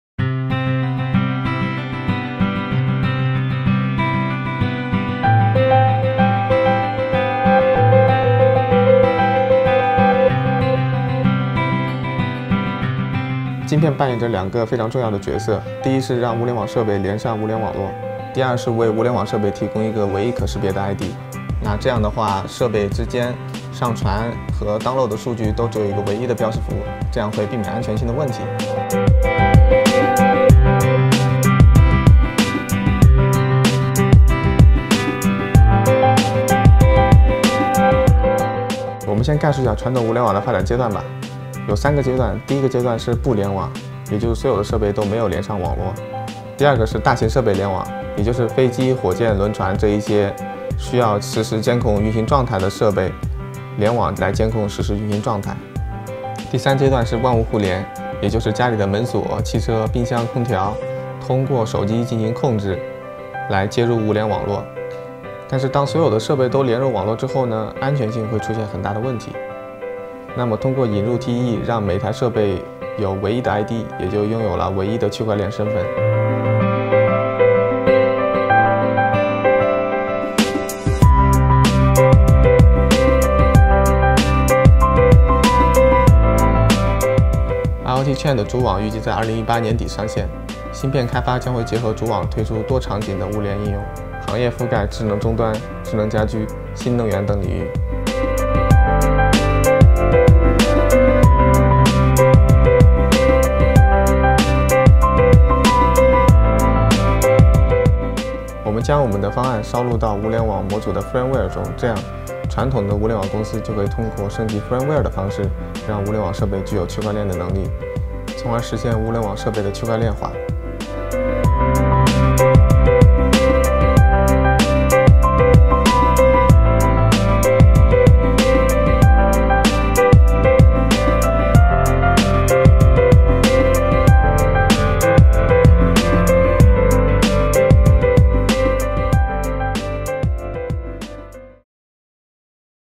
Introducing the IoT Chain chip
Chips play two significant roles in the blockchain, which are to connect devices to the Internet of Things (IoT) and to provide a unique ID for each IoT device.

Check out the current progress of the IoT Chain in chip development!